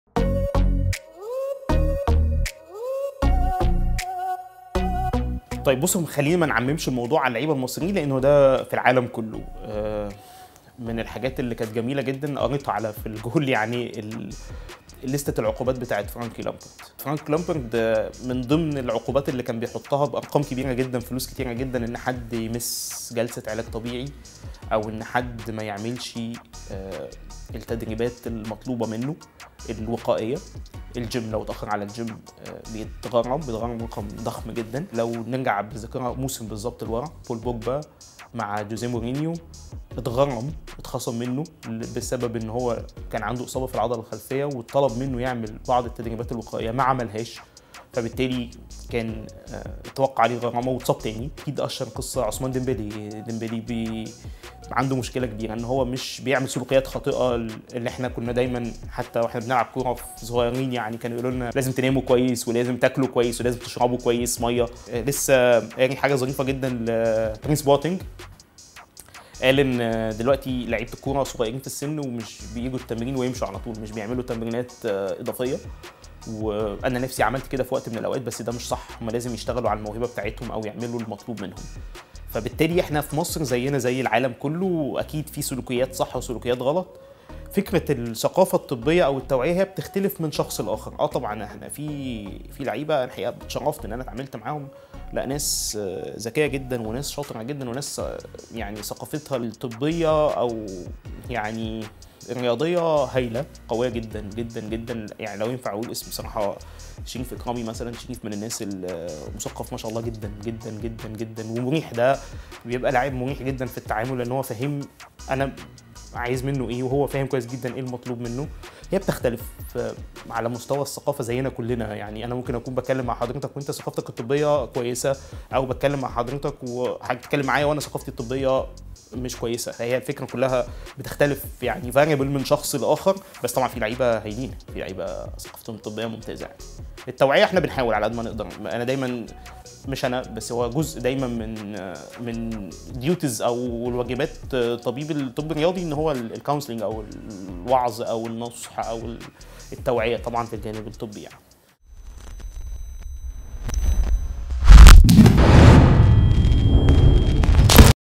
طبيب منتخب مصر يكشف هل يتسبب قلة الوعي الطبي في تأخير عودة اللاعبين المصريين من الإصابة
أي لاعب في تشيلسي يتاخر عن المواعيد الطبية يُعاقب
مورينيو وقع عقوبة مالية ضخمة على بوجبا بسبب تخلفه عن تدريباته الوقائية

محمد أبو العلا طبيب منتخب مصر يكشف لـ في الجول هل يتسبب قلة الوعي الطبي في تأخير عودة اللاعبين المصريين من الإصابة🤕